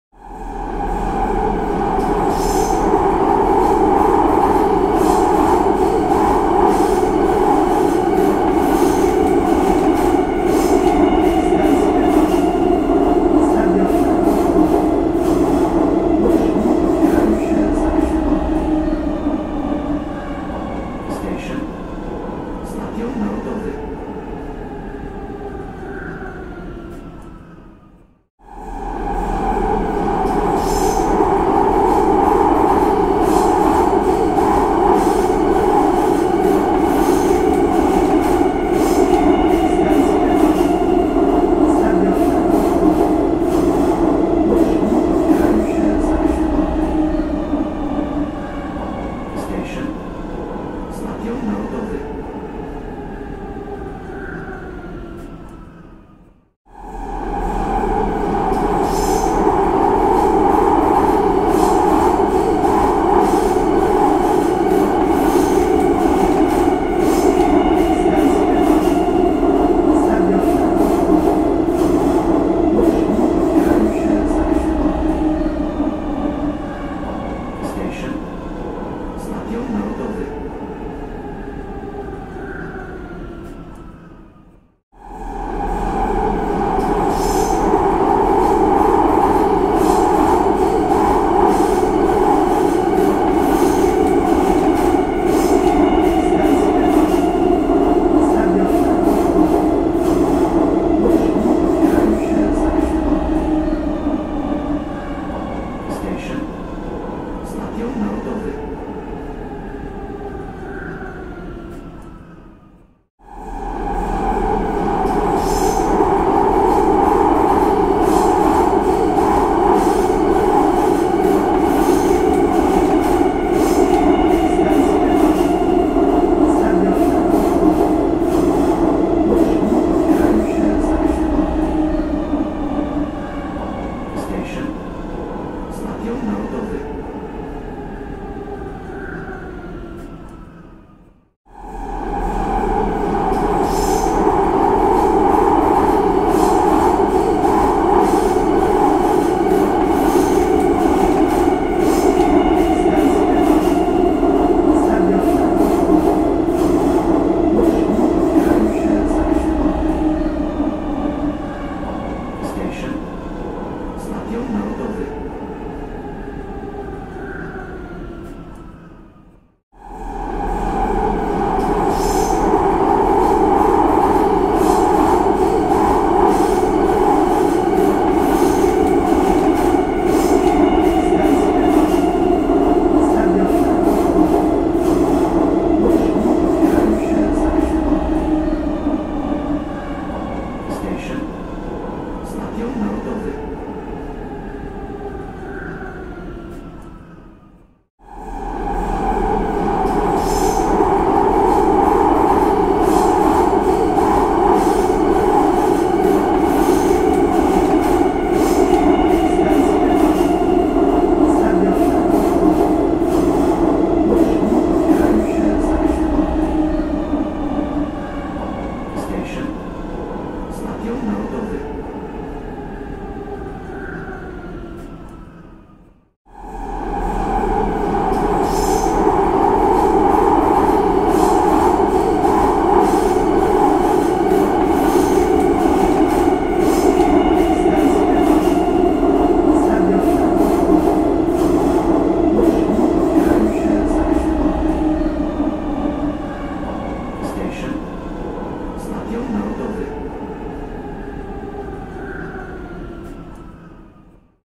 Subway Sound Effect
The interior sound of a rolling subway as it speeds over tracks through an underground tunnel. Metro underground station from the platform with train moving.